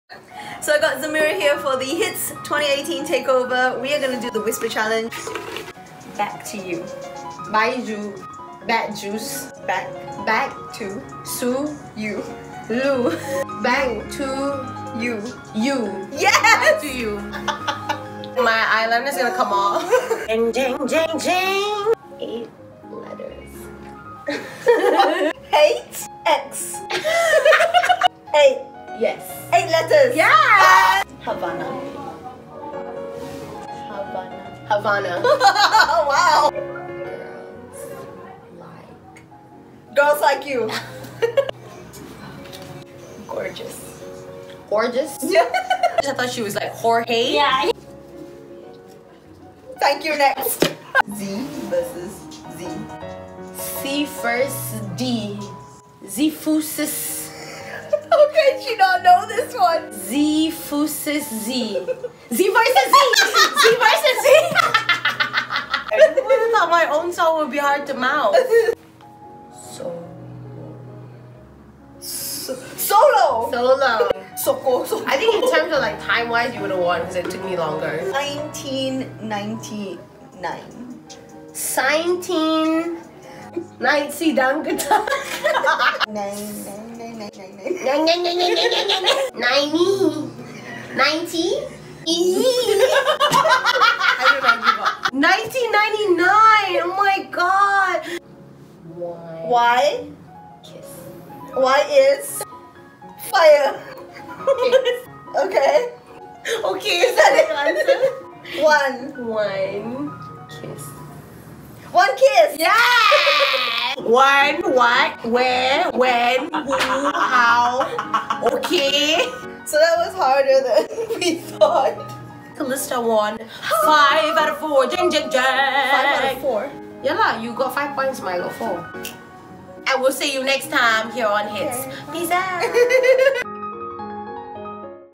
Calista Can’t Stop Laughing Playing The Whisper Challenge With Zamaera
Calista played the WhisperChallenge with Zamaera and can't stop laughing...